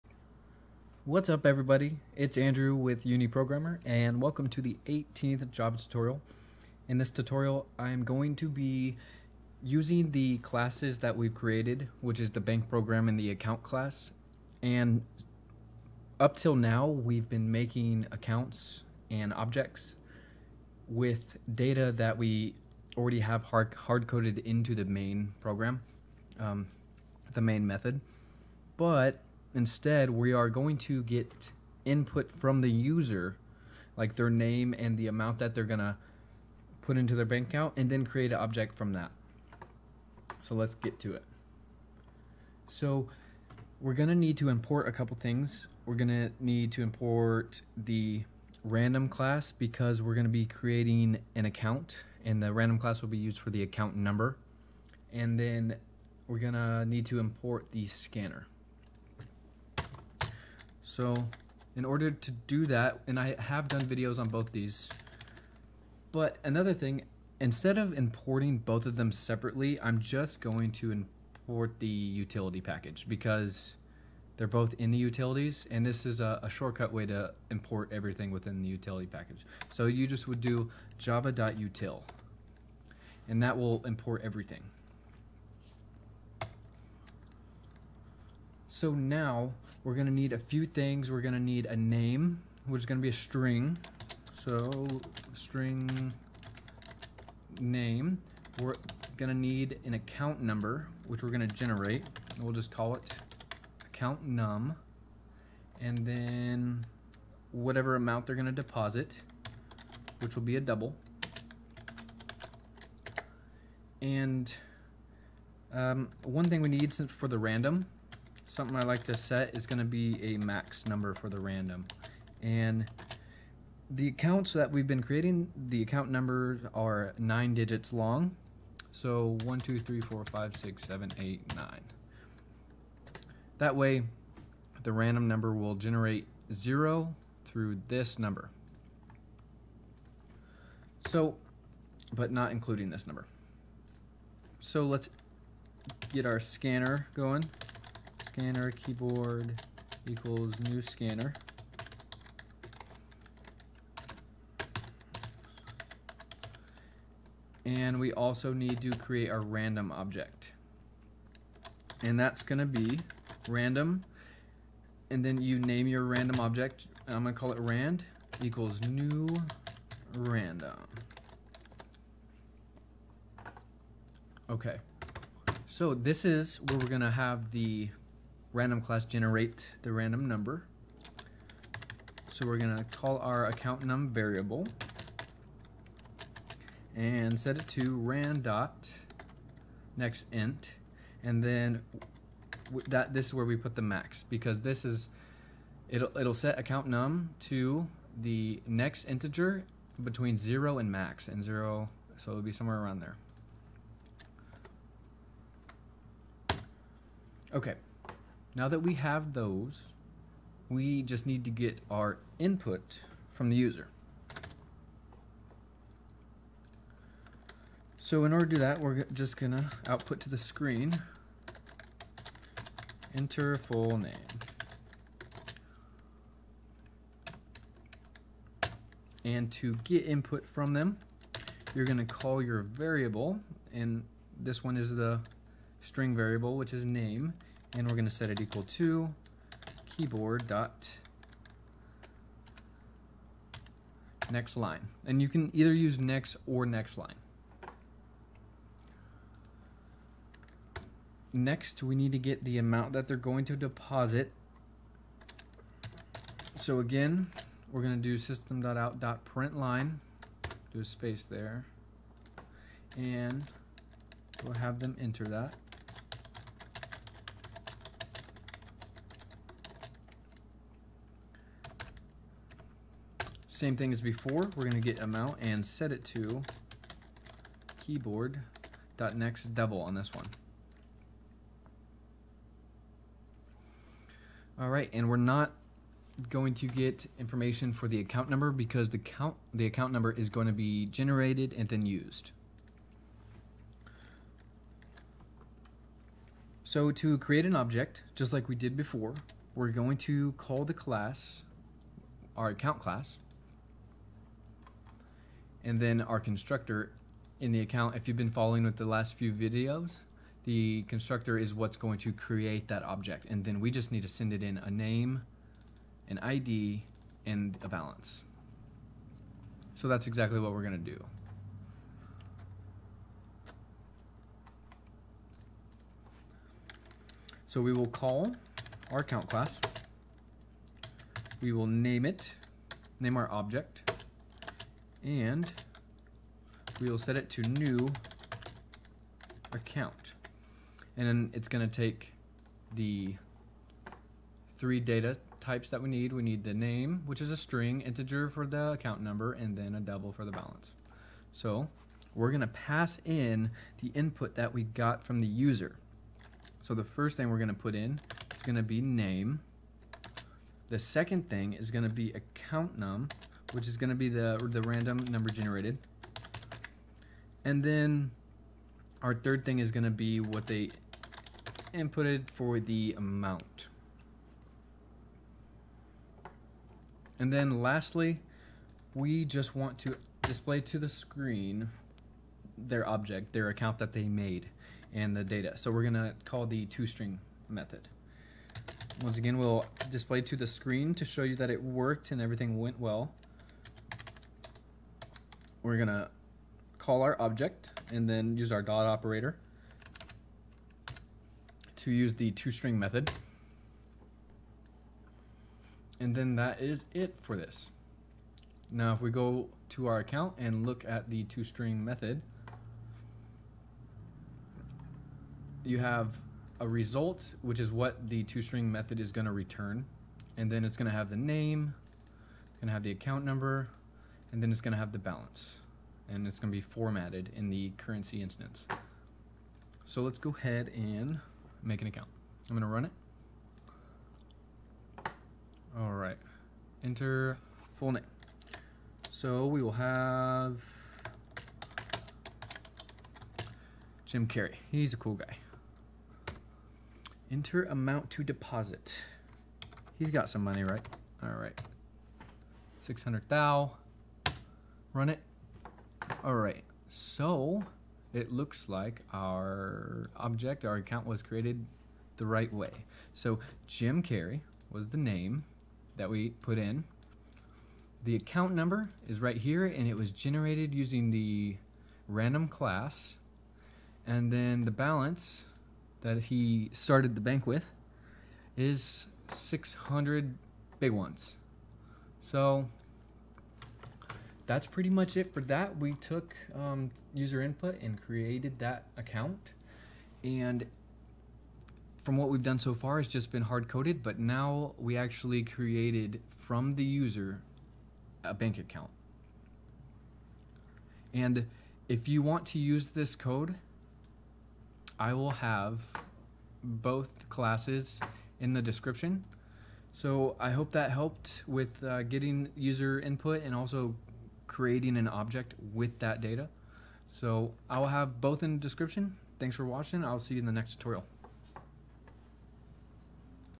Java Programming Tutorial - 18 - Getting user Input and Creating Objects
Main Class
import java.util.*;

public class BankProgram {

  //variables used to create account
  String name;
  int accountNum;
  double amount;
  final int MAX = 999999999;

  //Scanner for user input, Random for random number.
  Scanner keyboard = new Scanner(System.in);
  Random rand = new Random();

  //random number generated for account number. generates a number from 0 up to MAX.
  accountNum = rand.nextInt(MAX);

  //get user input to create account
  System.out.println("Enter full name: ");
  name = keyboard.nextLine();
  System.out.println("Enter amount to deposit: ");
  amount = keyboard.nextDouble();

  //instantiate new object, acct1, with user's values
  Account acct1 = new Account(name, accountNum, amount);

  //display account information using toString method
  System.out.println(acct1.toString());

Account Class
import java.text.*;

public class Account {

 //instance variables
 private String name;
 private int accountNum;
 private double balance;

 //the Account constructor
 public Account(String initName, int initId, double initBalance){

  name = initName;
  accountNum = initId;
  balance = initBalance;

 //method to deposit a specified amount into the account
 public void deposit(double amount){
  balance = balance + amount;

 //method to withdraw a specified amount from the account
 public void withdraw(double amount, double fee){
  balance = balance - amount - fee;

 //getter method to return balance
 public double getBalance(){
  return balance;

 //toString method that returns the accounts information
 public String toString(){
  String result = "";
  NumberFormat fmt = NumberFormat.getCurrencyInstance();
  result = "\nName: " + name + "\nAccount Number: " + accountNum + "\nBalance: " + fmt.format(balance);
  return result;